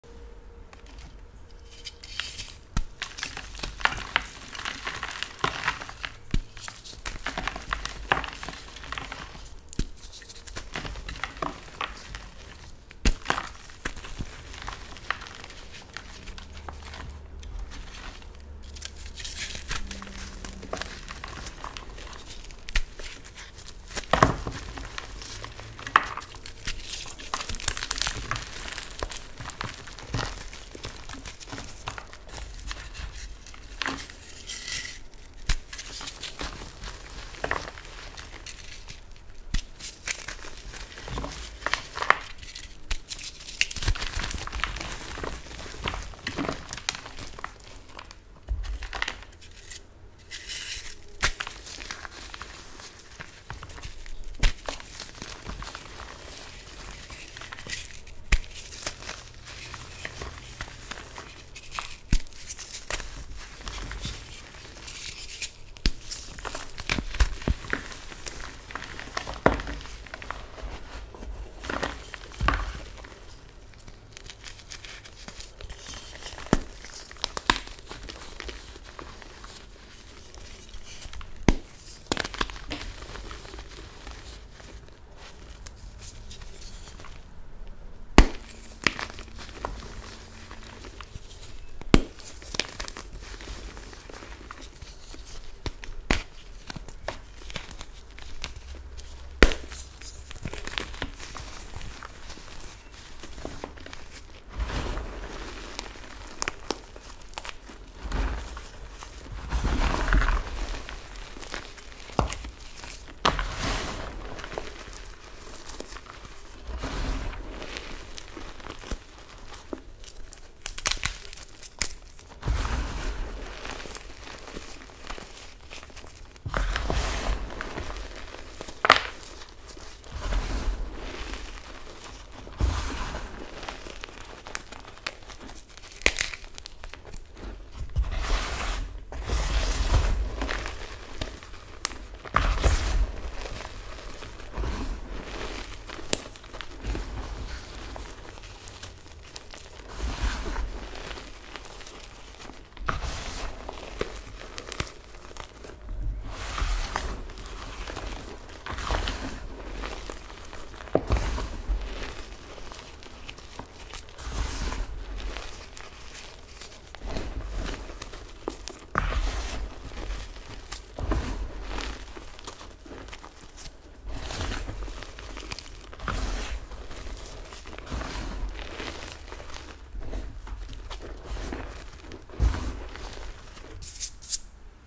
Gym Chalk Blocks Crush | Fresh & Reforms
Hello, friends! In this crush we have 4 gym chalk block reforms & 1 fresh gym chalk block topped with glitter. I hope you enjoy this crush. 😊